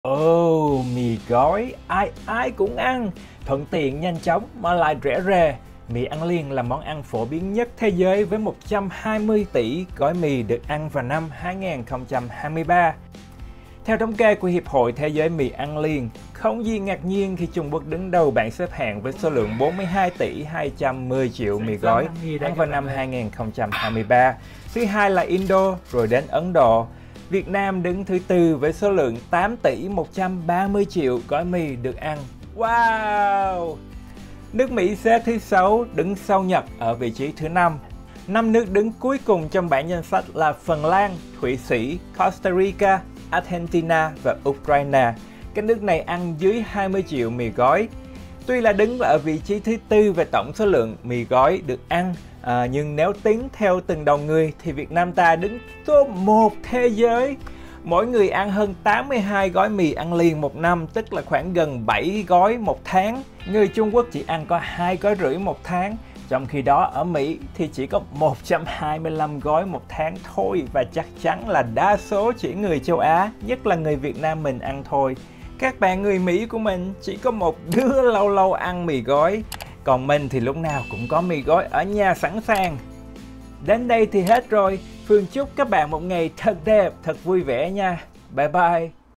Mì Gói Bá Cháy! Ai Ăn Nhiều Nhất? Xếp Hạng Các Nước! Việt Nam Mình Ăn Dữ Ghê! Ở Mỹ Có Ăn Không?
Cùng Phương khám phá các con số thí thú về món mì gói ăn liền nha.
Thông tin là từ đây nếu các bạn muốn tìm hiểu thêm